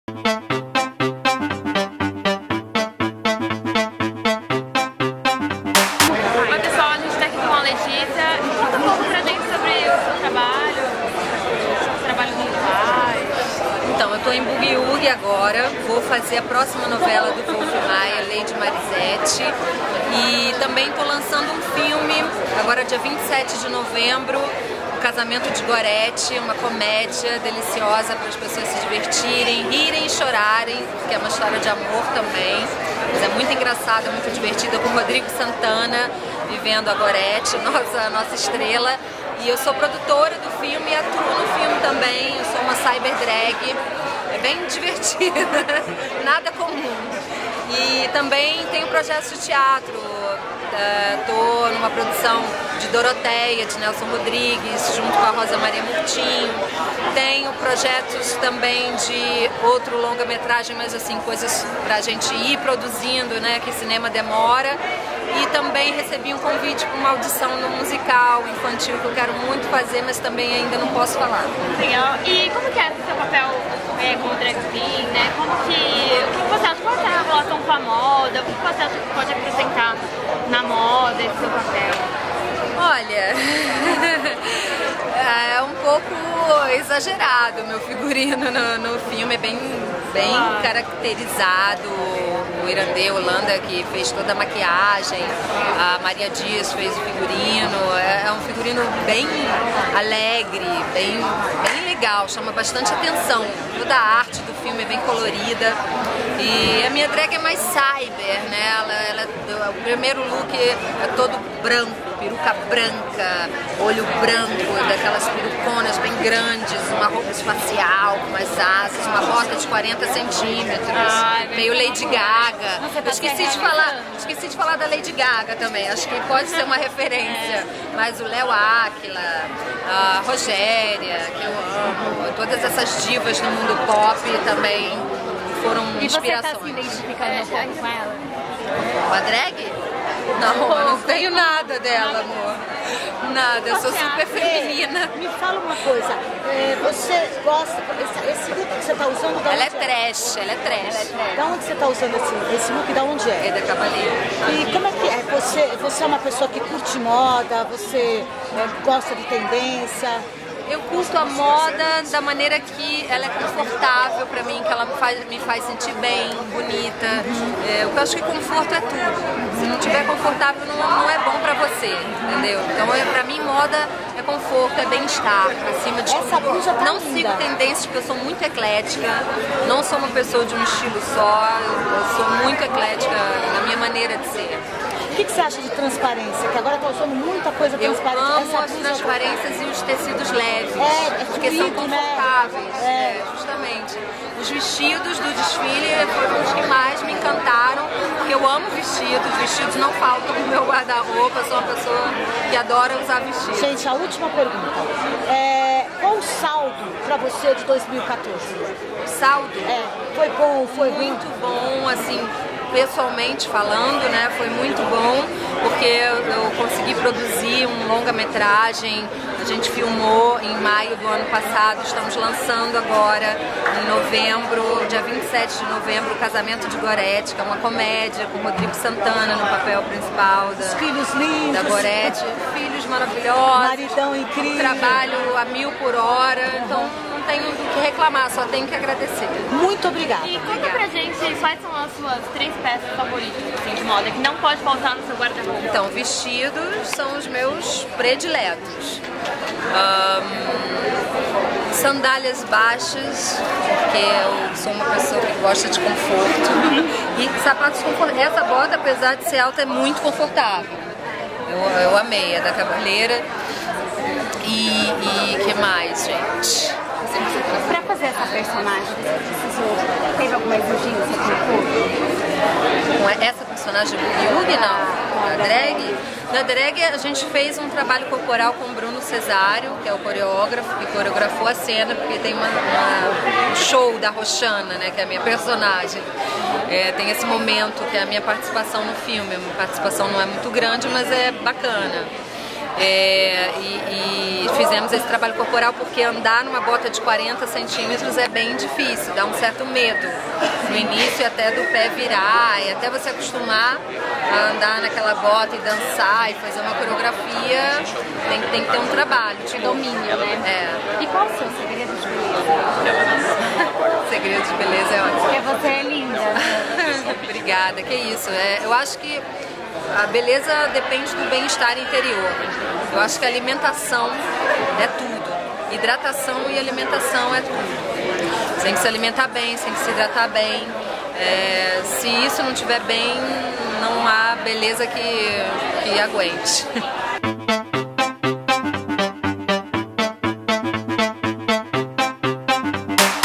Entrevista :: Leticia Spiller
Letícia Spiller conta tudo sobre sua nova personagem Drag Queen, fala sobre seus próximos trabalhos e segredos de beleza! Ficou curiosa? Dá o play!
Acesse o blog Fashion Frisson para saber mais sobre Moda, Beleza e Lifestyle